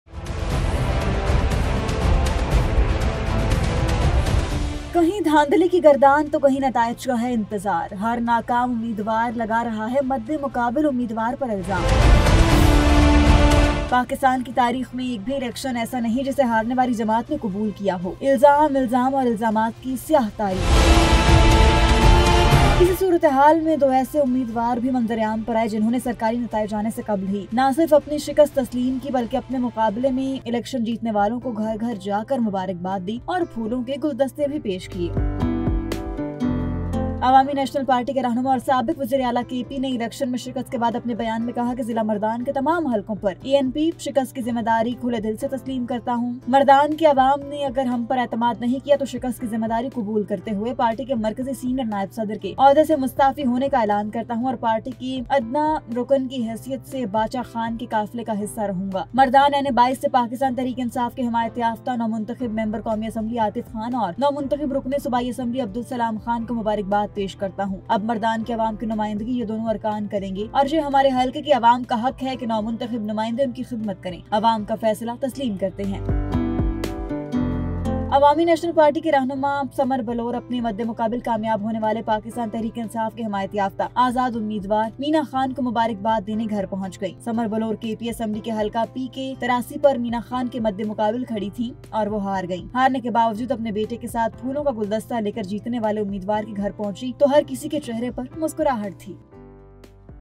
ANP Won Hearts Even After Losing The Election | Great Example Of Excellence In Khyber Pakhtunkhwa
ANP Won Hearts Even After Losing The Election | Great Example Of Excellence In Khyber Pakhtunkhwa | Aik News